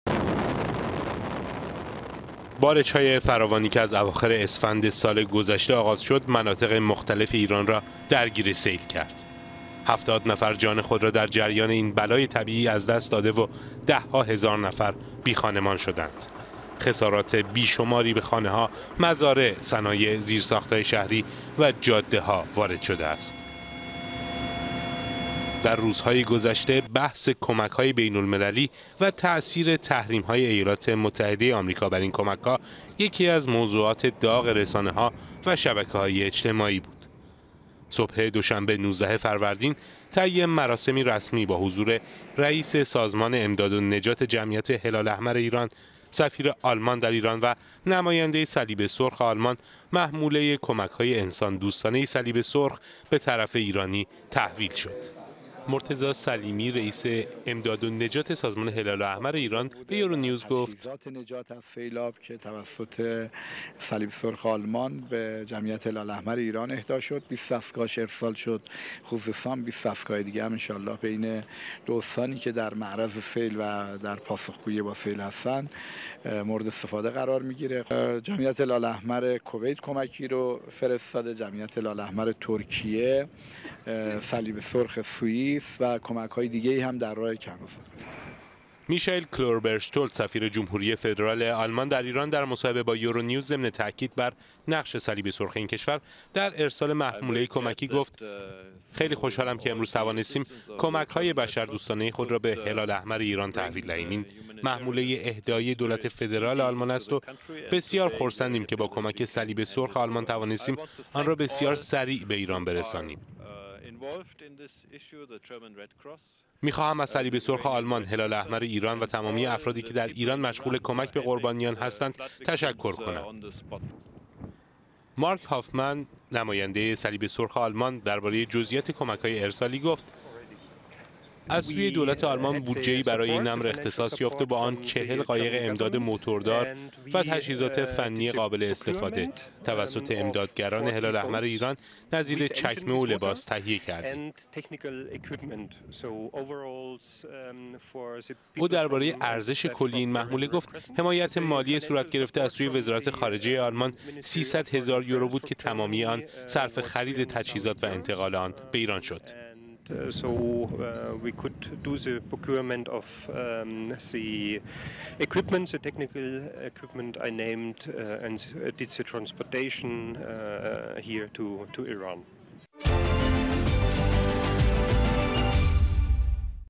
در غياب ديگر كشورهای عضو برجام، آلمان به یاری سیل‌زدگان ایران رفت…
با توجه به گستردگی دامنۀ آسیب ناشی از سیل و اوضاع وخیم سیل زدگان، کمک‌های بشردوستانۀ کشورهای خارجی، بویژه هم پیمانان برجام، به انتظاری عمومی تبدیل شده است.…